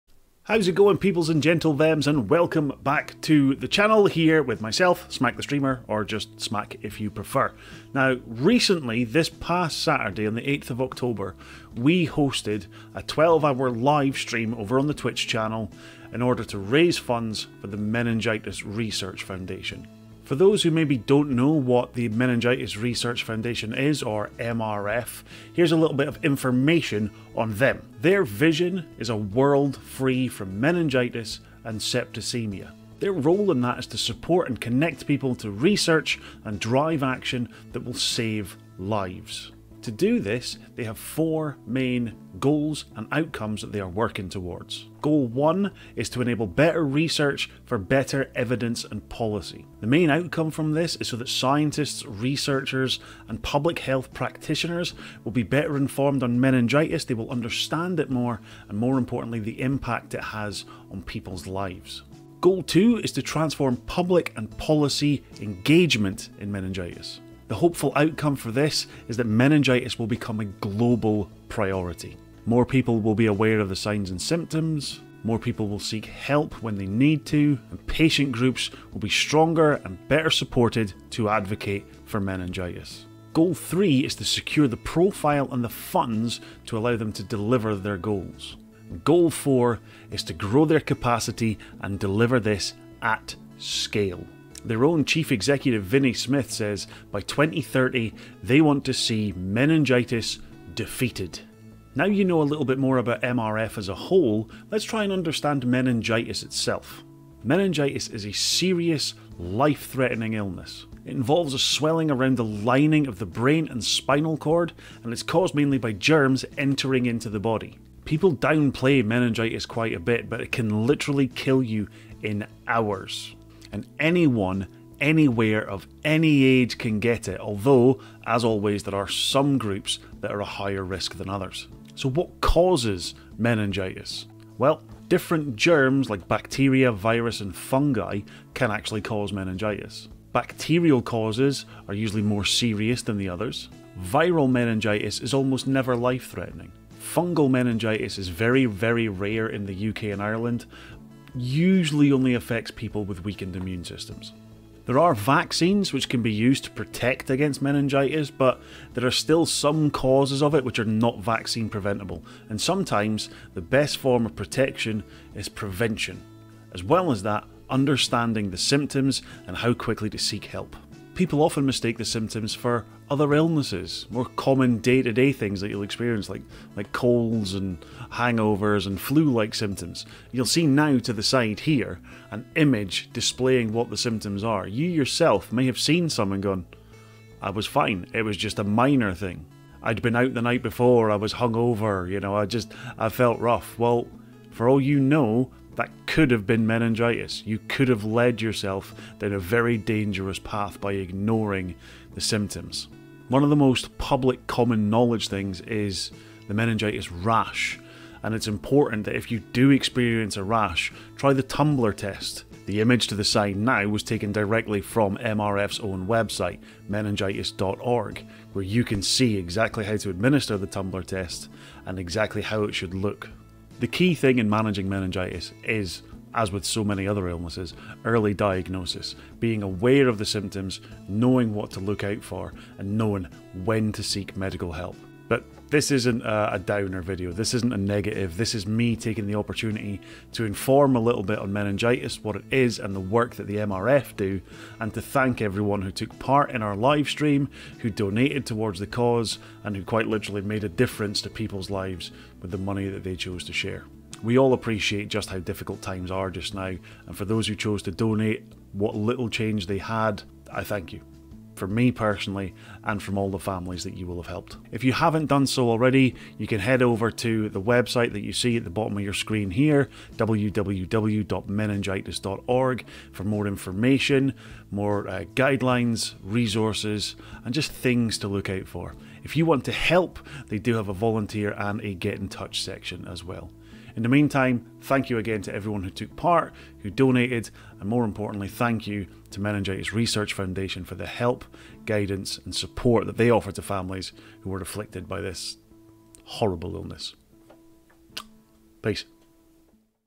MENINGITIS - Raising Awareness 2022
On October 8th, I hosted a 12 hour charity stream in order to raise funds for the meningitis Research Foundation. This is just a little video to go along with that and thank everyone who contributed/participated.